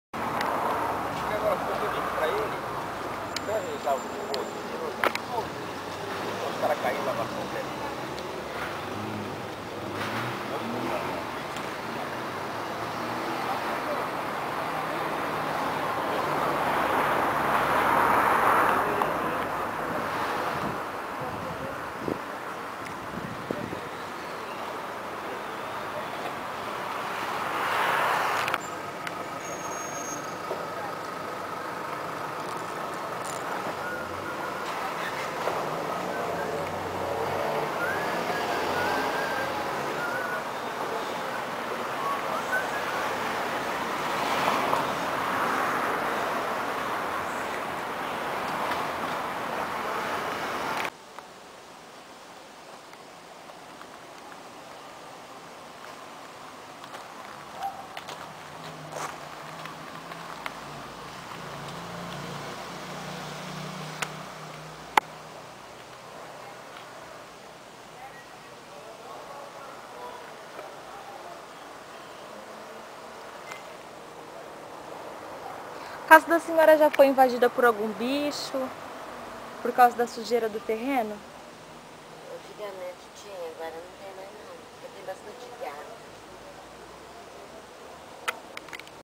Disque-Folha: Abandono de terreno gera reclamação.wmv
Um terreno abandonado na rua Bandeirantes, Centro, tem causado preocupação. O comerciante Ariceu de Souza Carvalho, 55, disse que a área está coberta por mato e sujeira, e não passa por manutenção há vários meses, possibilitando a proliferação do mosquito da dengue e o surgimento de animais peçonhentos.